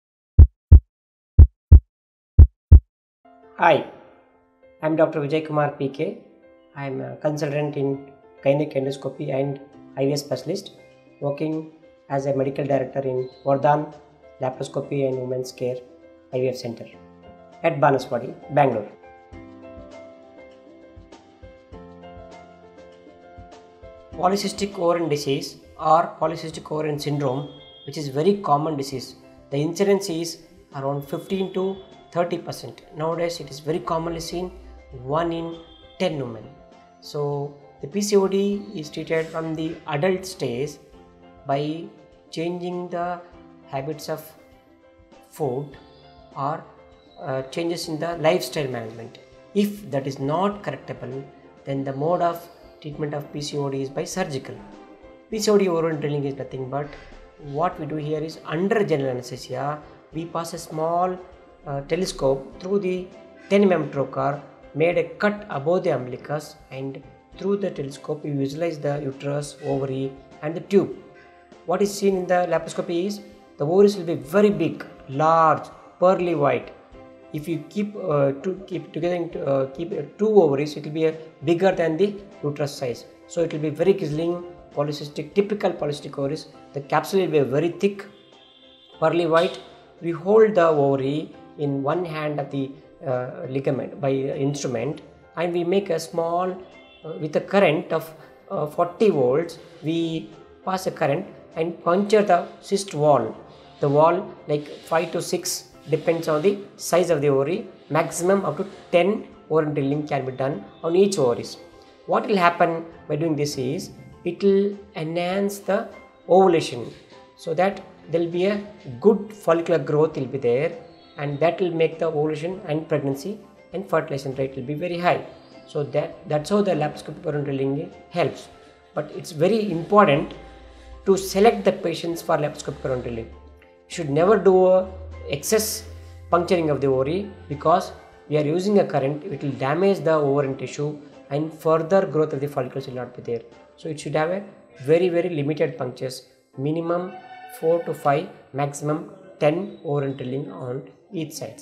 Use of Laproscopic Ovarian Drilling in PCOD - Dr. Vijaykumar P K
Poly-cystic ovaries or poly-cystic ovarian syndrome which is very common disease, the incidence is around 15 to 30%.  Now a days it is very commonly seen in one in ten member. So the PCOD is treated from the adult stage by changing the habits of  food or changes in the Lifestyle management if that is not correctable then the mood of treatment of PCOD is by surgical. PCOD ovarian drilling is nothing but what we do here is under general anesthesia  we pass small telescope through the Tanium trocar made a cut 1above the umbilicus and through the telescope we visualize the uterus ovary and tube what is seen in the Laparoscopic is the most is a very big large pearly white if you keep two ovaries it will be bigger than the uterus size, so it will be very killing Polycystic typical ovary the capsule will be very thick curly white, we hold the ovary in one hand knee of the ligament by instrument and we will make a small with the current off 40 voltage we pass a current and puncture the cyst valve, the valve like 5 to 6 depends on the size of the ovary maximum up to 10 ovarian drilling can be done on each ovary what happens is it will enhance the ovulation. So that will be a good follicle growth I'll be there and that will make the origin and pregnancy and fertilization will be very high. So that's how the laparoscopic ovarian drilling helps, but it's very important to select the patience for laparoscopic ovarian drilling, should never do excess puncturing of the ovary because you are using the current it is damaged ovary and tissue and further growth of the Fallopian will not be there, so it should have a very limited punctures minimum 4 to 5 and maximum 10 on ovarian drilling on each side.